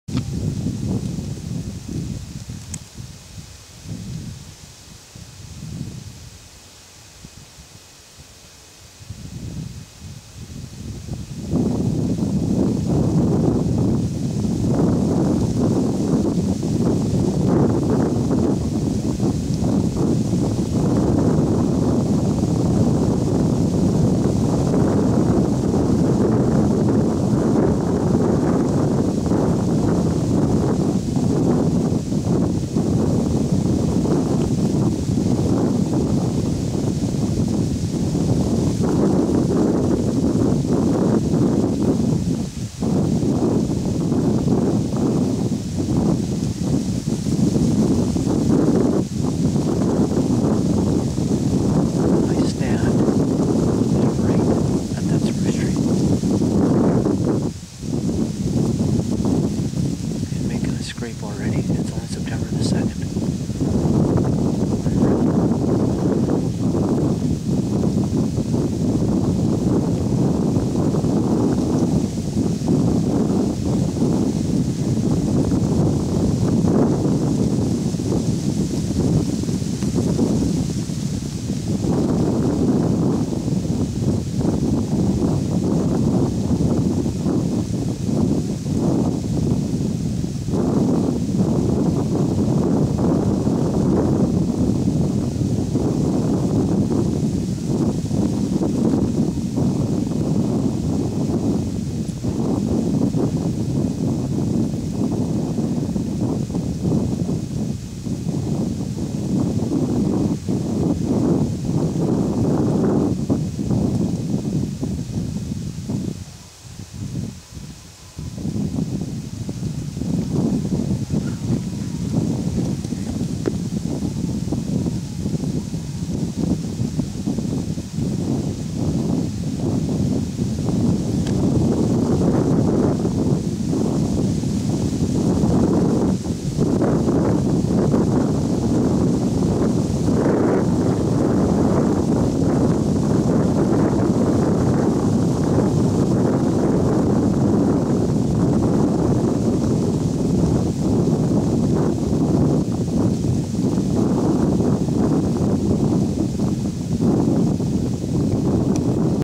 Young Buck Sept 02
I have trail camera pics of this young buck 2 days prior in full velvet. 2 days later and he is marking his territory already. Little does he know I have a tree stand 15 yards to the right of where he is making his scrape. Hope he gets lots of visitors.